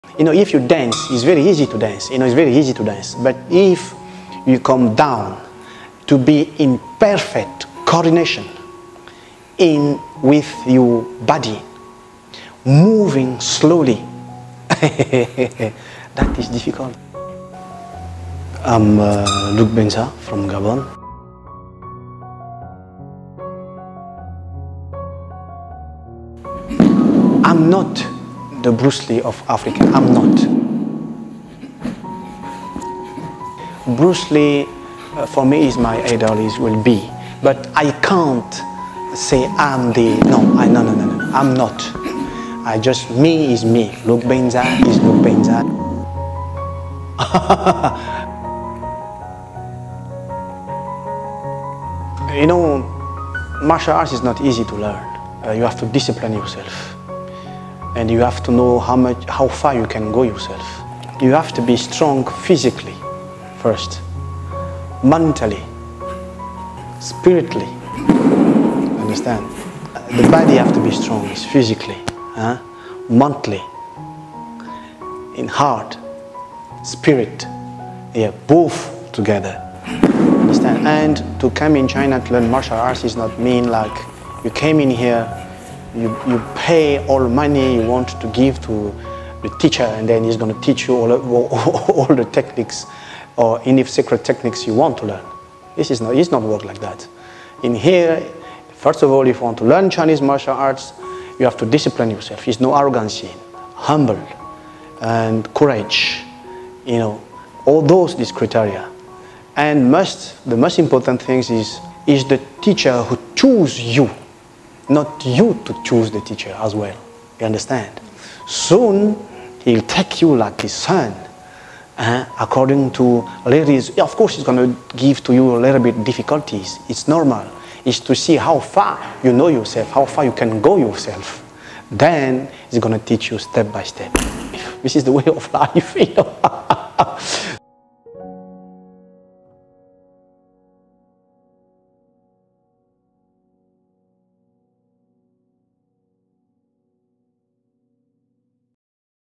Luc Bendza -The African 'Bruce Lee'.
He's been dubbed the African Bruce Lee.  Meet Gabonese martial arts star Luc Bendza who moved to China nearly 40 years ago to achieve his Kungfu dream. In this video, he talks about what it takes to qualify not only as a martial arts student but also what it takes to succeed at the craft.